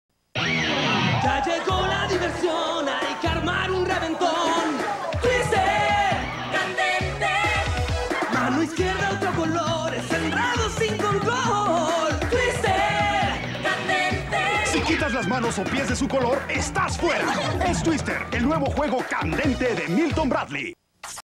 Twister Ad - The Hardest One 1 (1999 Mexico)
A cool new ad that I have been searching for for quite a long time. I bet an even older version exists.